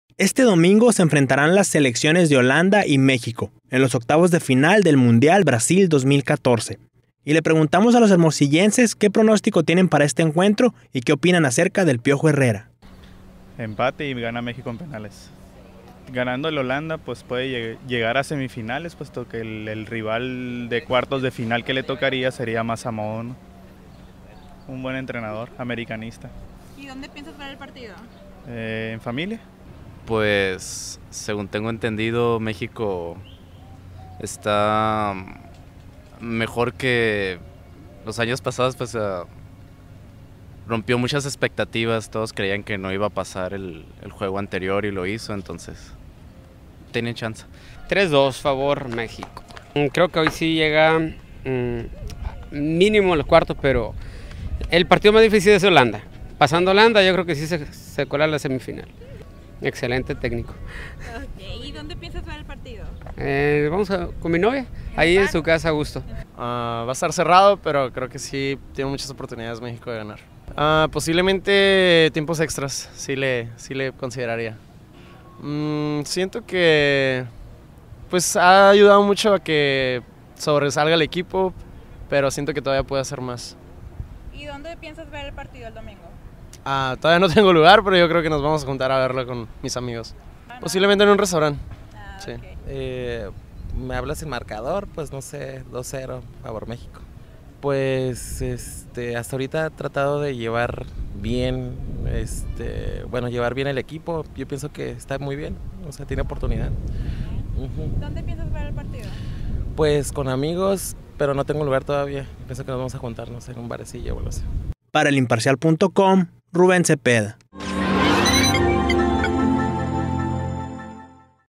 Opinan hermosillenses del México vs Holanda
Este Domingo se enfrentaran las selecciones de Holanda y México en los octavos de final del Mundial Brasil 2014, y le preguntamos a los hermosillenses que pronostico tienen para este encuentro y que opinan acerca del Piojo Herrera.